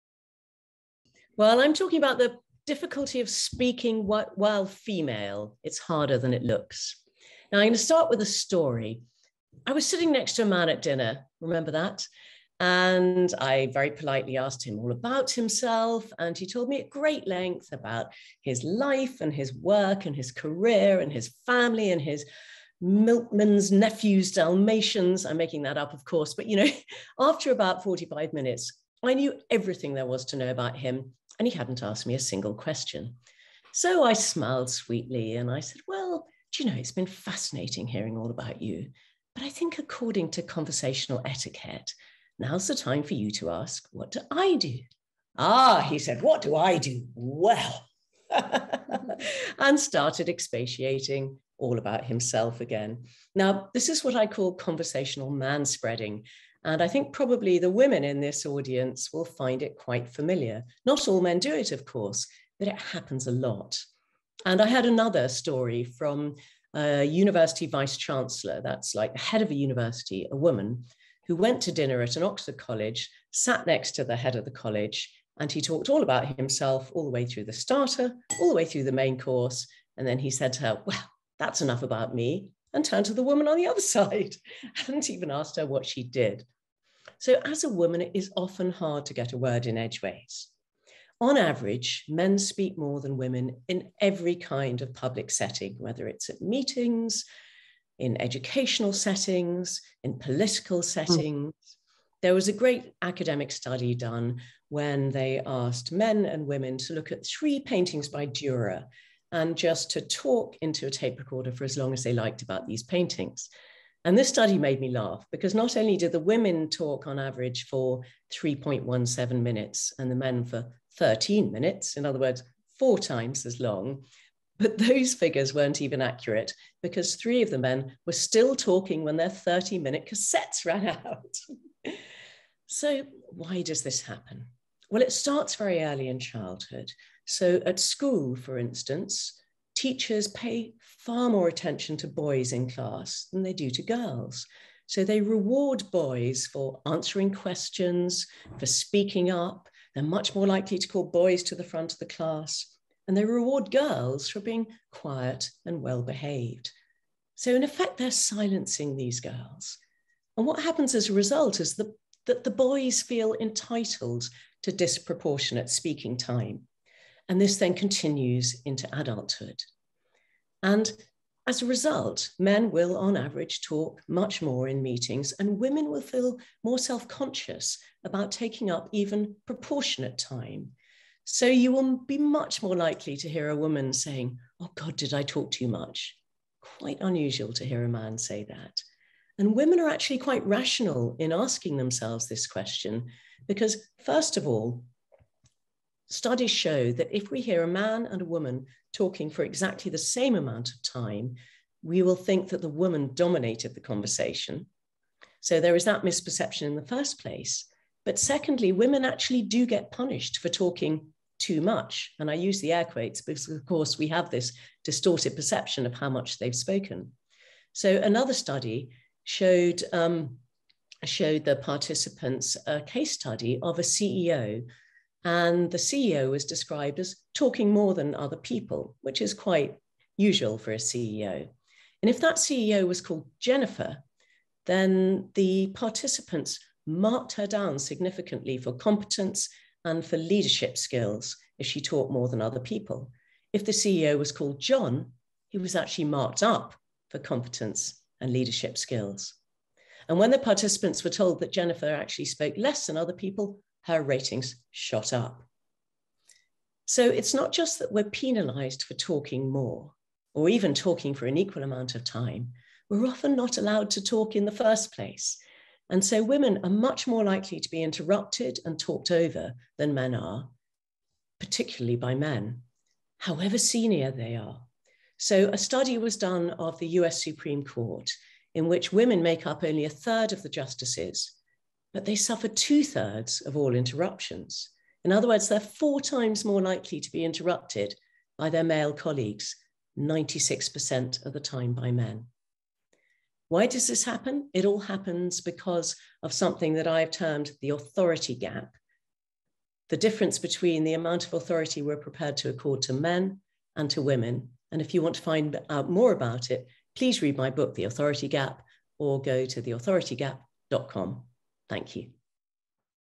Could you please just let me fini-? - Mary Ann Sieghart
Speaking while female is harder than it looks